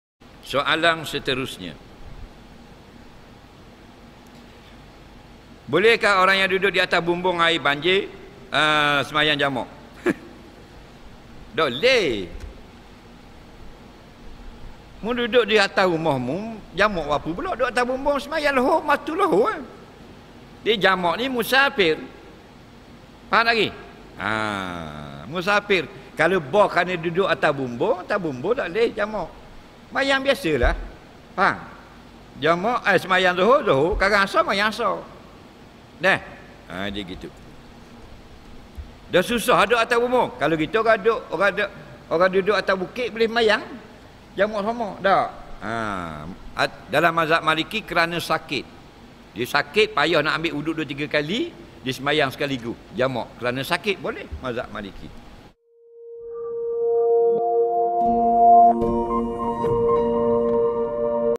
Bolehkah Orang yang Duduk Atas Bumbung Air Banjir Buat Solat Jamak? - Ustaz Azhar Idrus
Kuliyah Maghrib Jemputan Ustaz Azhar Idrus Official

🕌  Masjid Nur Bakti, Gong Badak, Terengganu
📆 23 Disember 2021
⏰ 07.30 malam